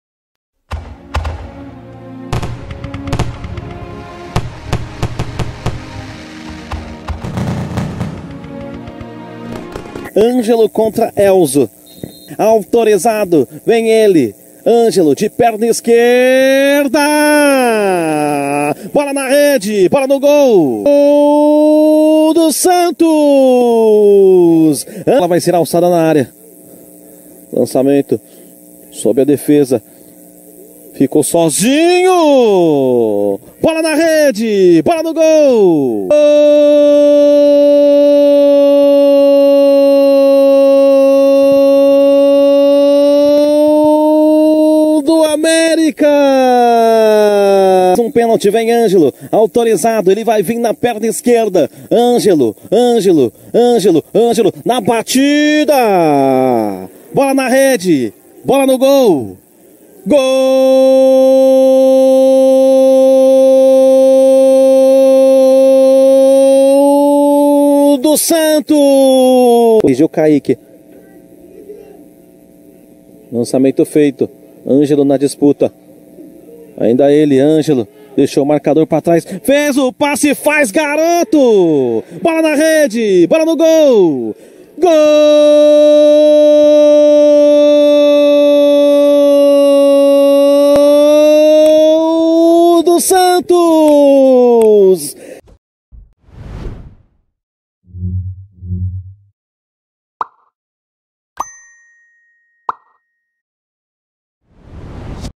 Brasileiro Sub-20 / 2020 - Santos Vence o América por 3 a 1
O Santos venceu o América-Mg por 3 a 1 pela 4ª rodada do Brasileiro Sub-20 de 2020 jogando praticamente com sua equipe Sub-17, Gols de Ângelo (2) e Nycollas para o Santos e Carlos Alberto descontou para o América.

Santos 3x1 América
Local: CT Rei Pelé. Gols: Ângelo (2), Carlos Alberto e Nycollas Lopo. Cart. Amarelos: Gabriel Oliveira e Goldeson. Cart. Vermelhos: Gabriel Oliveira.

América
Elzo, Einsten, Gustavo, Carlos Junio, Gustavo Vieira (Vinicius Ventura), Matheus Santos, Diogo (Gabriel Alves), Gustavinho, Lucas Gabriel (Goldeson), Carlos Alberto e Kawê (Pedro Henrique). Técnico: Paulo Ricardo

Santos
Breno Sossai, Sandrinho, Yalle, Kaiky Fernandes, Gabriel Oliveira, Kevin Malthus, Rafael Moreira, Gabriel Gabú, Ângelo (Hyan), Brayan (Negueba) e Nycollas Lopo (Heron). Técnico: Rodrigo Chip.